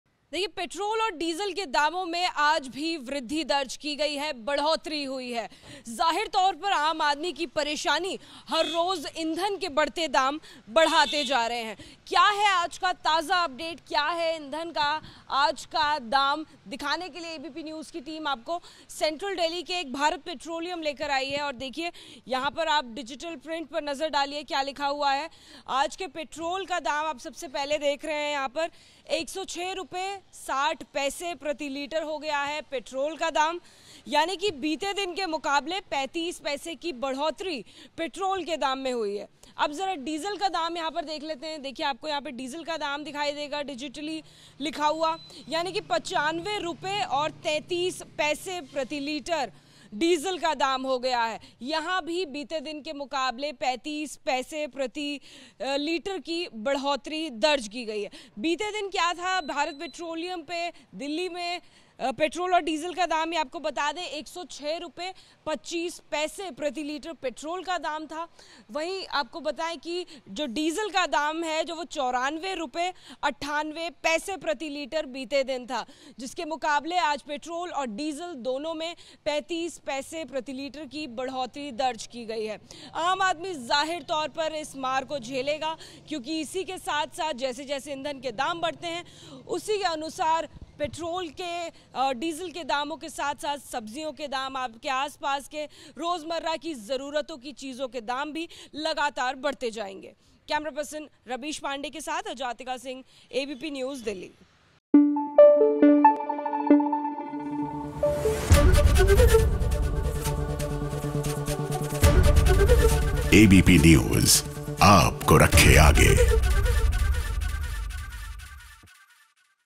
बढ़ोतरी के बाद आज Delhi में कितनी है Petrol-Diesel की कीमत? | Ground Report
Petrol Diesel Price Today: देश में हर दिन पेट्रोल-डीजल के दाम बढ़ रहे हैं और आम आदमी की कमर तोड़ रहे हैं. आज एक बार फिर वैश्विक कच्चे तेल की दरों में उछाल के बाद ईंधन की कीमतें अब तक के नए उच्चतम स्तर पर पहुंच गईं हैं.  राष्ट्रीय राजधानी में पेट्रोल और डीजल की दरें 35 पैसे बढ़ गई हैं.